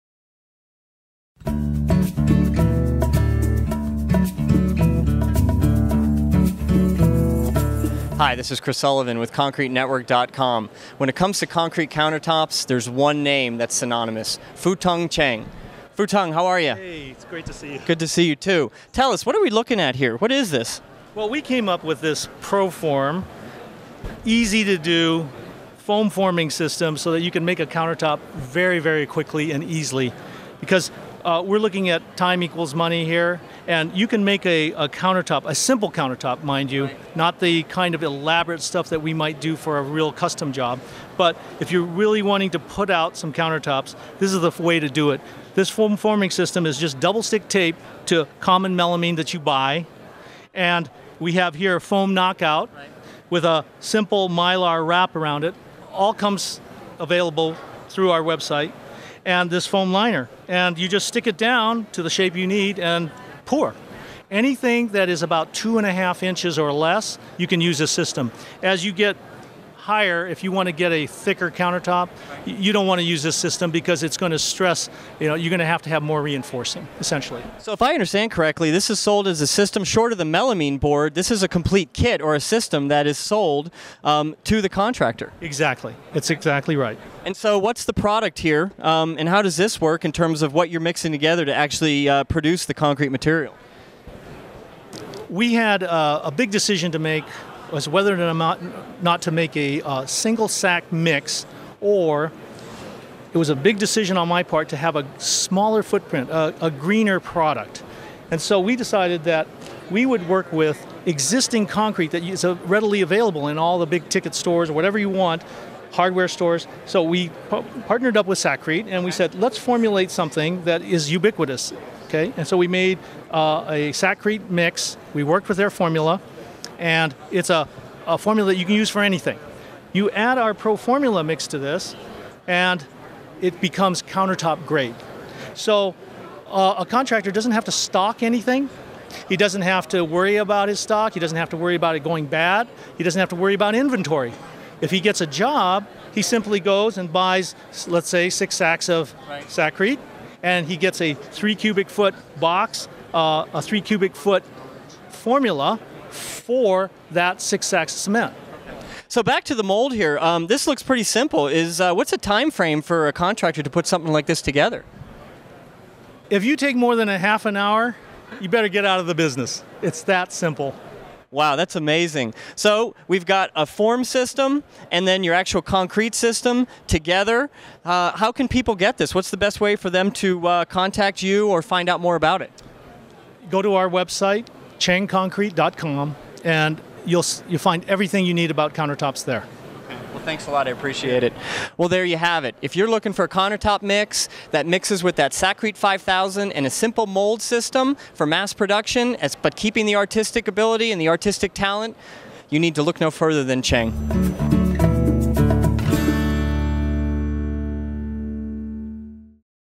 CHENG Concrete Pro-Formula Countertop Mix
Imagine being able to create a concrete countertop in just half an hour. For countertop fabricators who want to ramp up their production, thats now possible with CHENG Concretes Pro-Formula foam forming system, which comes complete with knockouts and form liners. When used with CHENG Pro-Formula countertop mix, which is added to sacked concrete along with water, the system makes producing concrete countertops easier than ever.